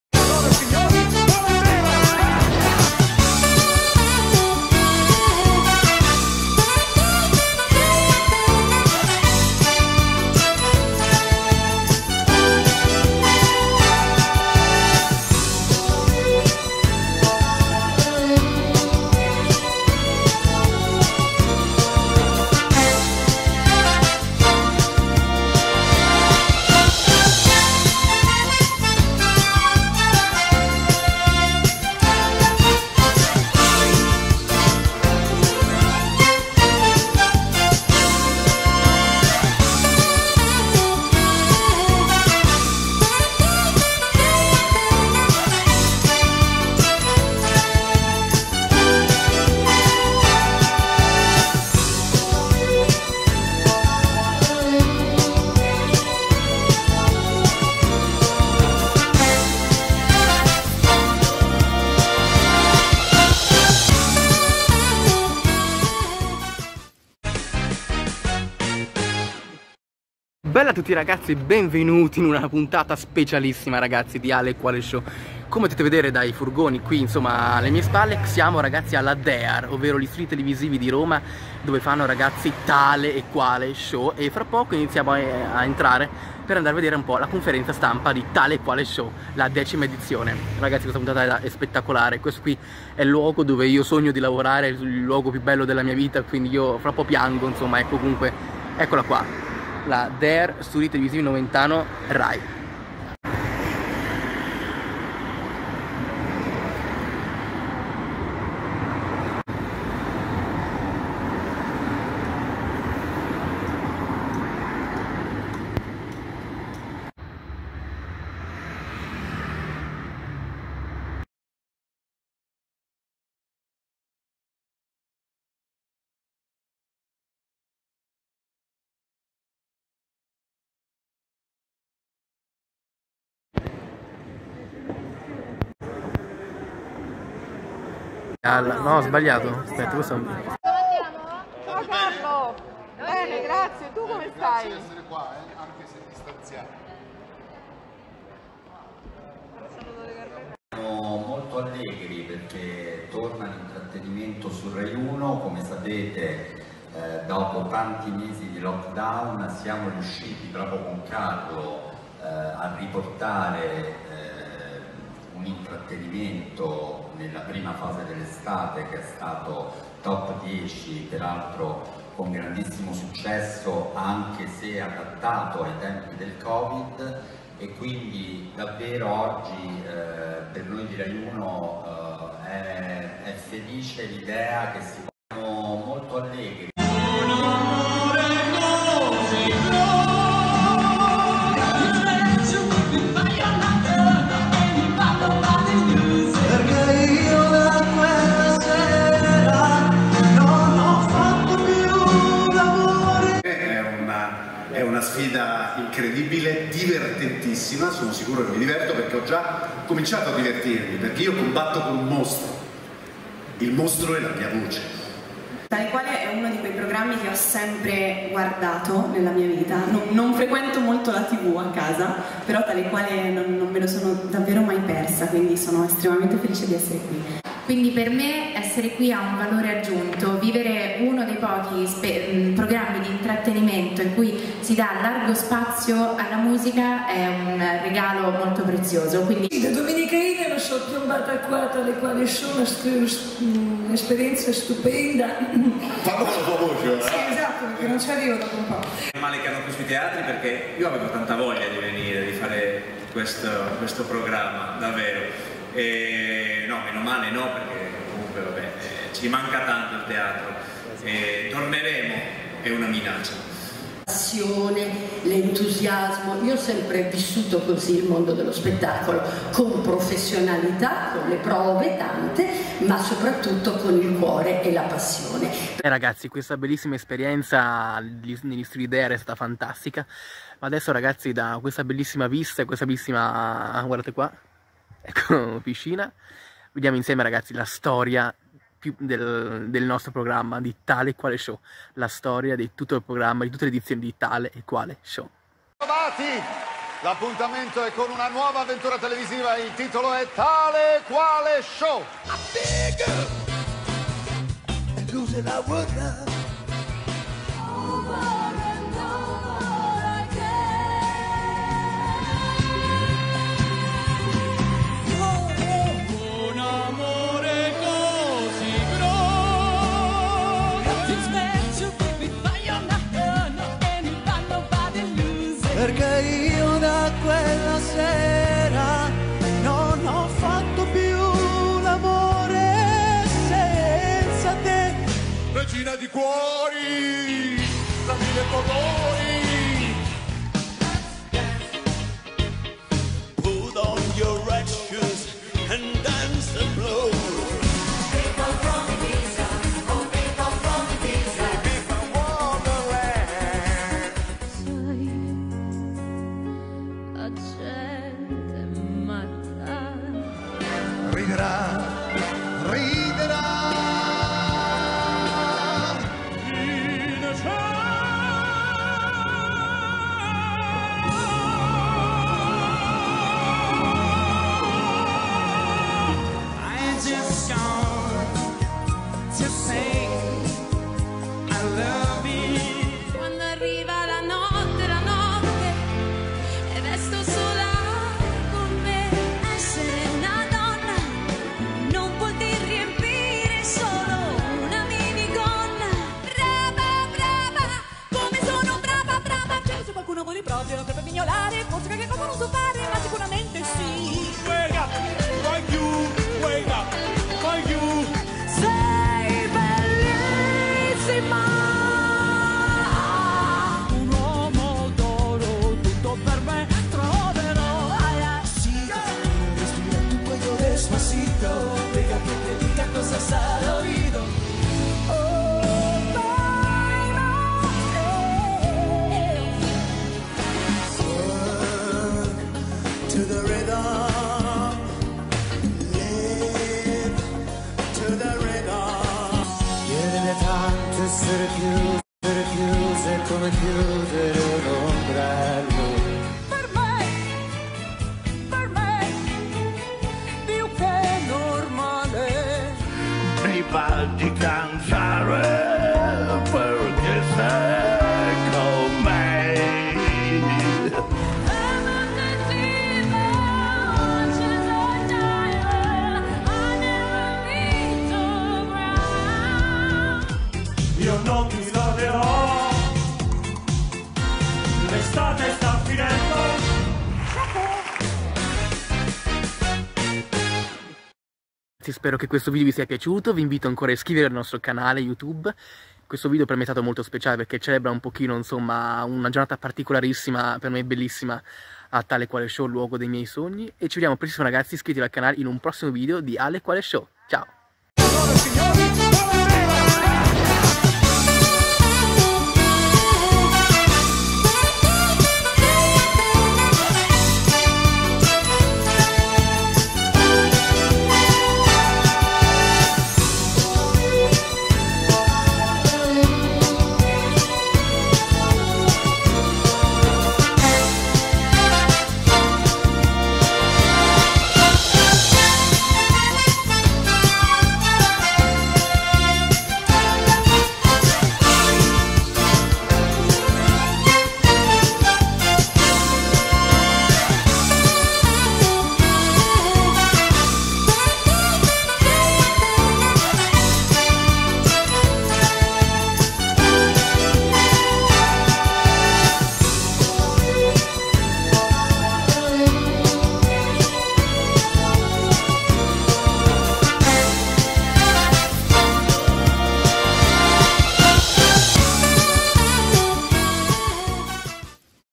TALE E QUALE SHOW 2020 - Conferenza Stampa , Dietro le Quinte e Storia del Programma
Settima puntata di Ale e Quale Show che vi porta alla scoperta del programma Leader di Rai 1 Tale e Quale Show giunto alla sua decima edizione .Attraverso una esclusiva conferenza stampa ed immagini inedite degli studi , Alessandro vi porta all'interno degli Studi Rai in un esclusivo dietro le Quinte! :D Buona Visione! :D